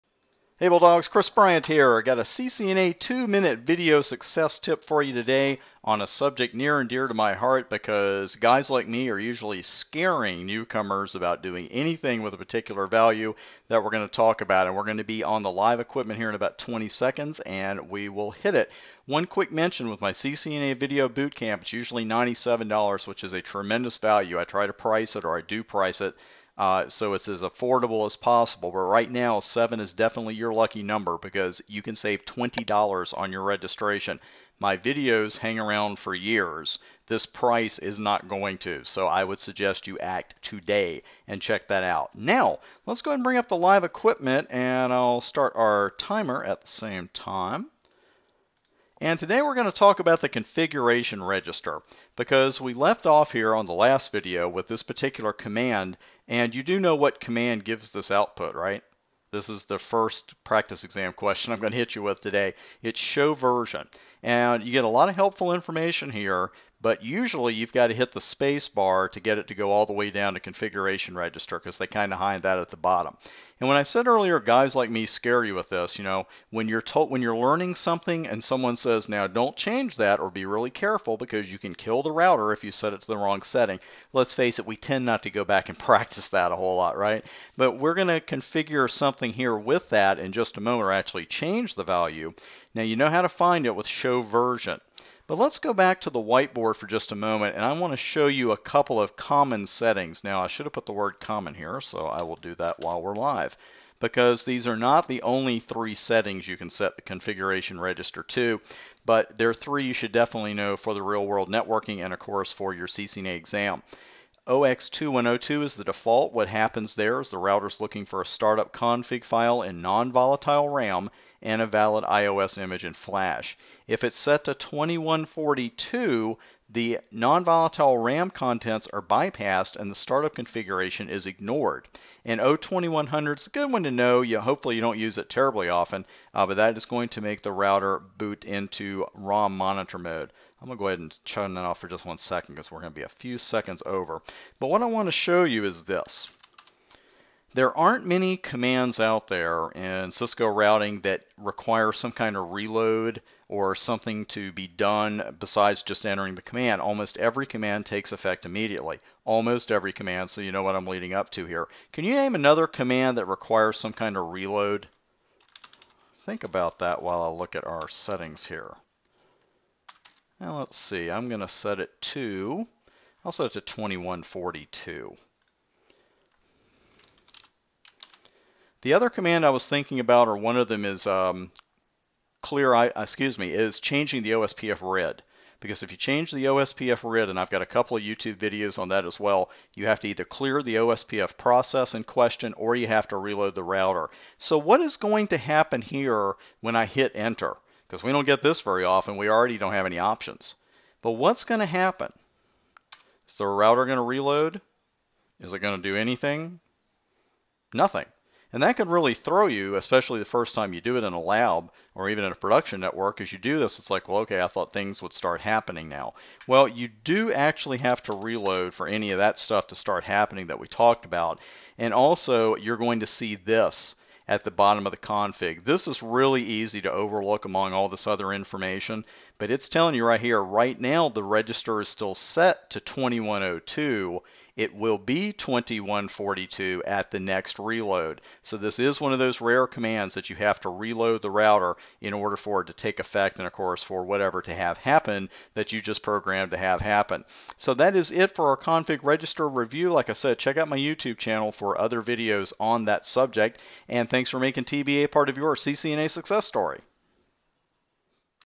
CCNA Exam Success Video: Config Register Tips
to get my 22-hour CCNA Video Boot Camp for more CCNA 3:00 Video Boot Camps!

In today's CCNA Exam Success video, I'll show you how to view and change the config register settings - and a little "gotcha" you have to know to make this work in the real world!